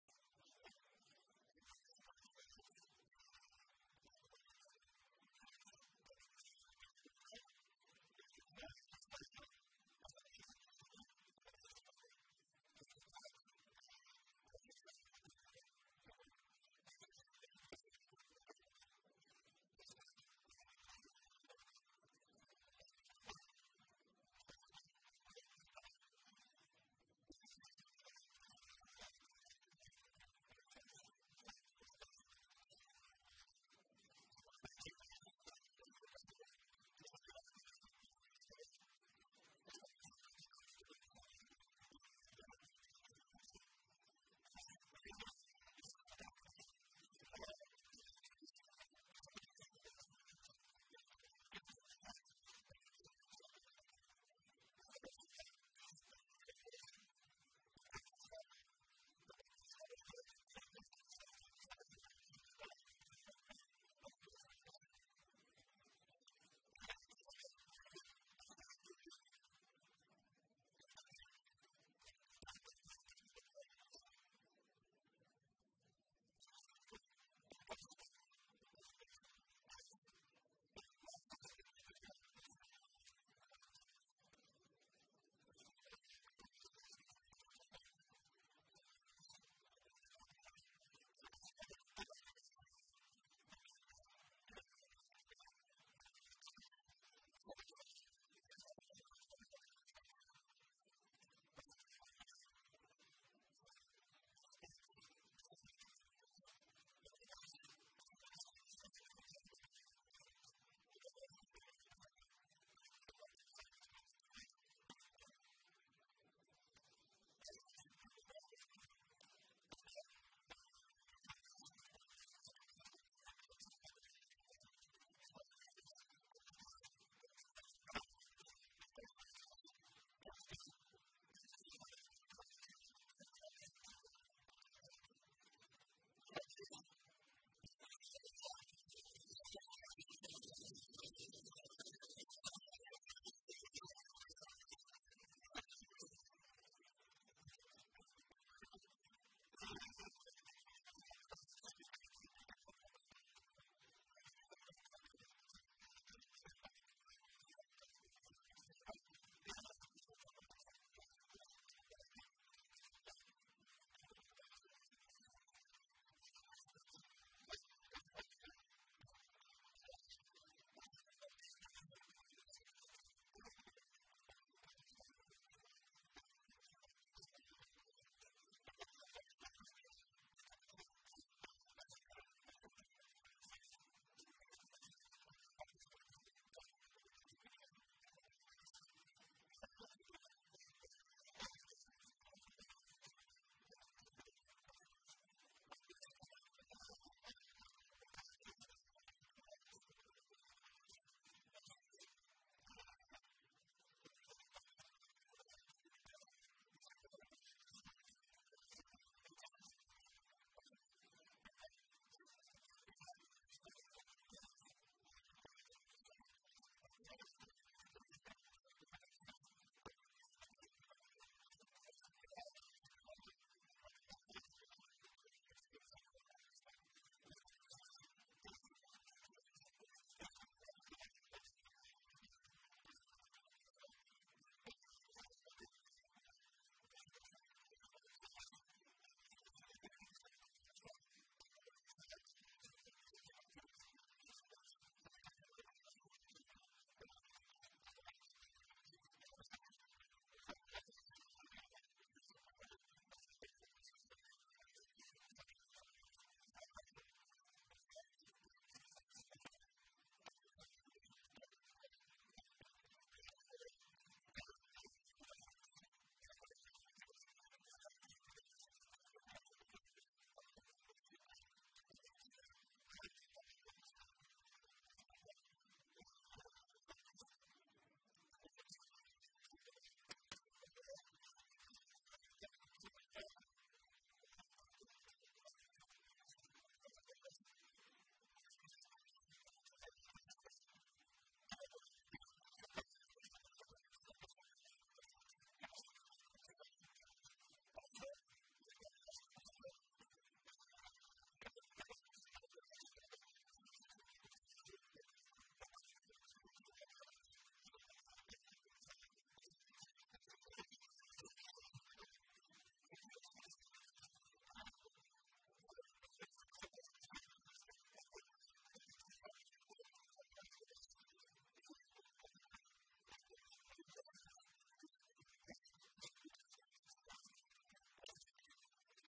Mulet al PP: "Defensen les Diputacions per poder continuar delinquint"
Intervenció de Carles Mulet sobre una proposta del PP en "defensa" de les Diputacions Provincials.